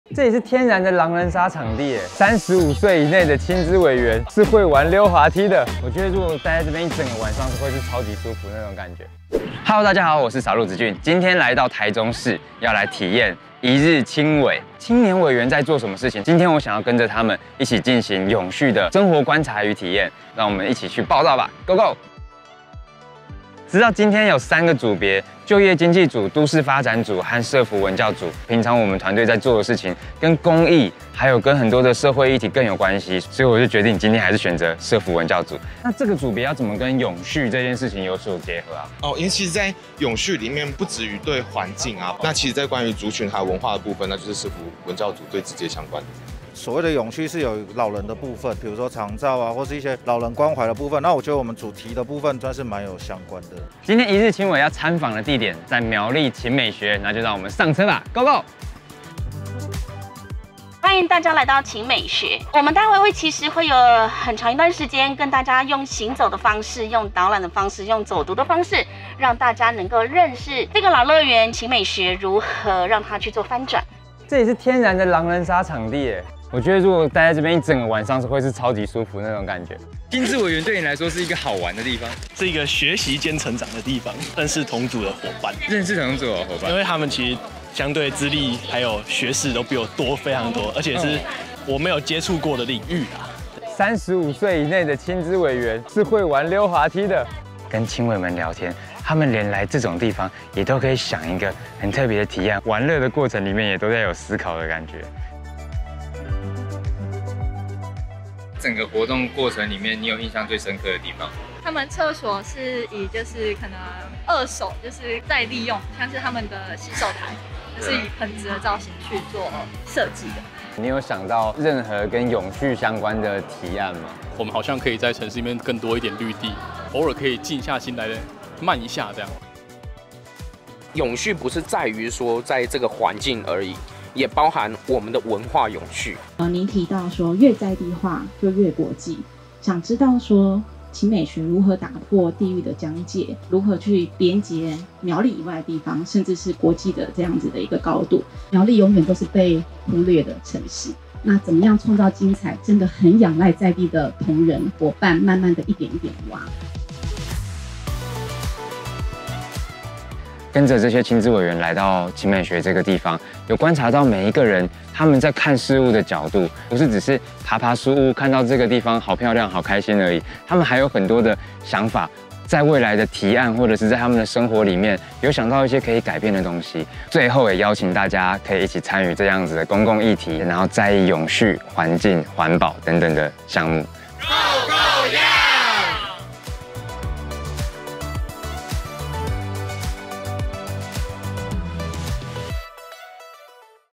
青年同行 開創未來GO GO YOUNG｜廣編企劃
2022年第二場臺中市靑年參與公共事務培力活動，將參訪臺中在地指標型企業勤美集團，位於苗栗永續實驗場域―勤美學。以鑄造業起家的勤美集團，傾力勾勒新生活場景的藍圖，從商場、美術館、傳統市場到豪華露營此起彼落、奪人耳目，從傳統製造走入各種新鮮的生活樣態中，呼應著當前時代的價値與追求。

勤美學串聯在地文化與產業鏈，整合產官學資源，媒合設計、藝術、生態、農業、飮食等不同領域的當代思維，創造出兼具國際高度與在地深度的臺灣新型態永續聚落 ！永續讓我們從生活做起！